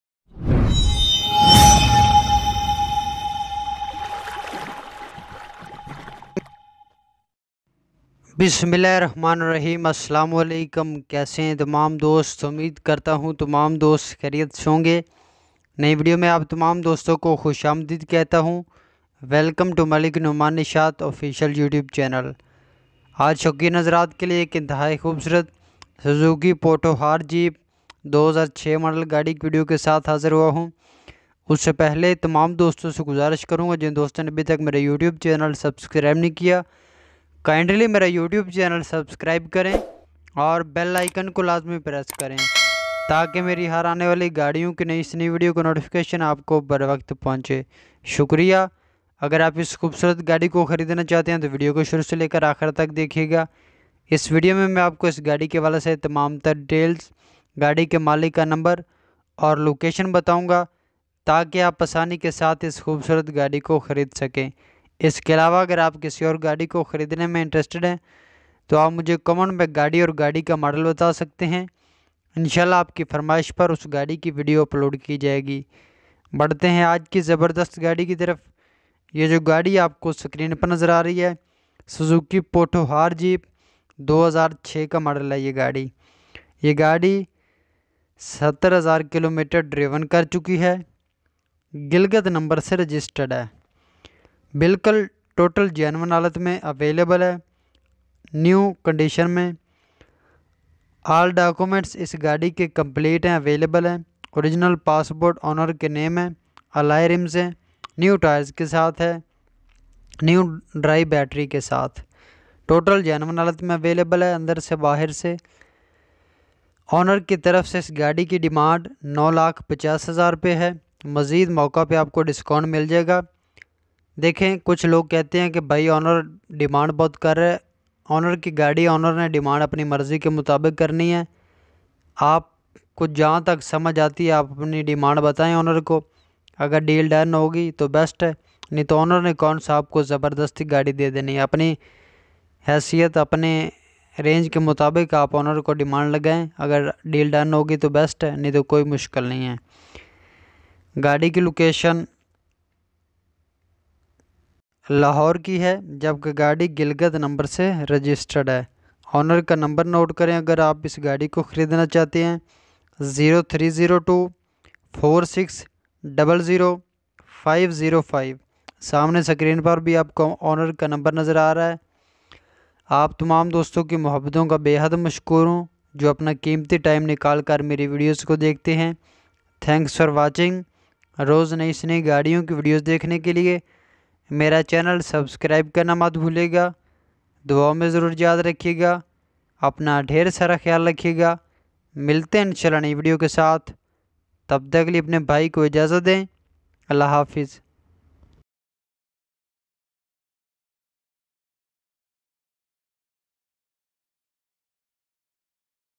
Suzuki Potohar Jeep | 2006 Model | Very Good Condition | Full Review
Location: Lahore